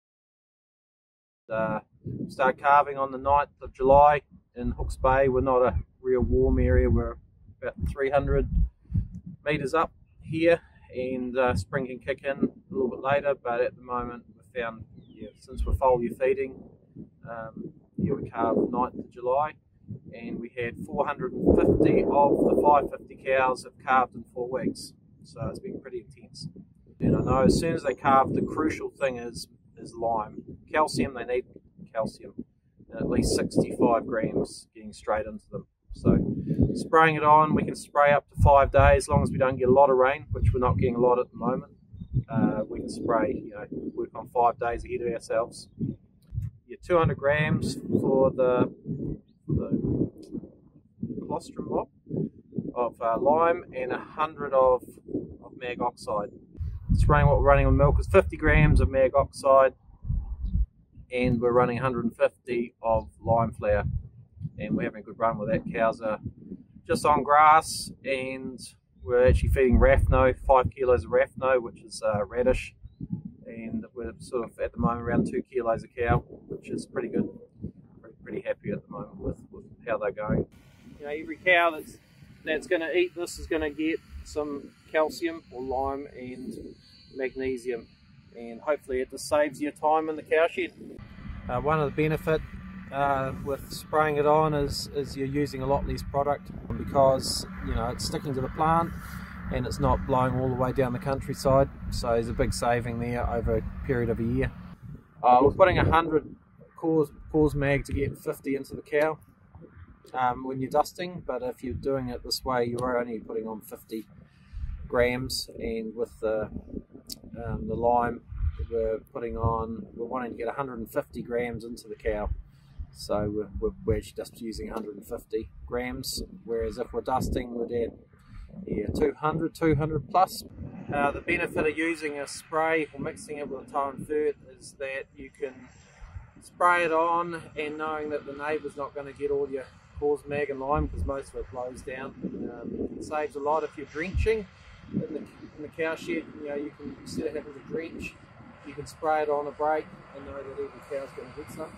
Tow and Fert useful at calving time
Apply mag oxide and lime flour at time of calving to get Calcium and Magnesium into the colostrum mob and the milkers!   Apply it the leaf of the plant in suspension to ensure it doesnt blow away and stays there for days in advance of grazing!   Apply approximately half of what you usually would due to guaranteed application and guaranteed uptake. The Tow and Fert is a versatile liquid foliar sprayer with quick return on investment.

Tow and Fert will dissolve your solid Urea in it’s tank, keep Ultra Fine Particle Fertilisers suspended in liquid and even allow you to spray weed spray and the small seeds your pasture needs all in one pass, saving you money spent on fertiliser, fuel and tractor maintenance.